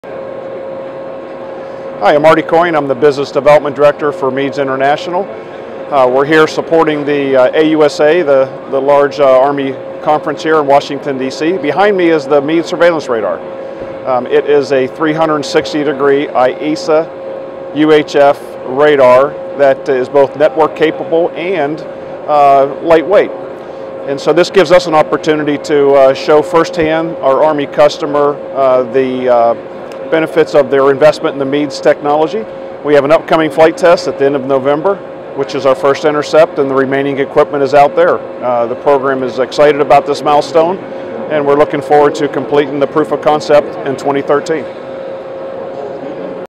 MEADS at AUSA 2012
A brief video of MEADS from the AUSA show floor.